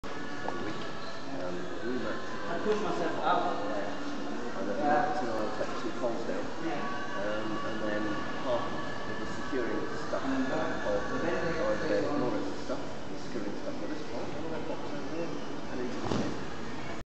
Chinese Pole Move Demonstration
Moritz Linkman Chinese Pole Class Demonstration. Glasgow June 2011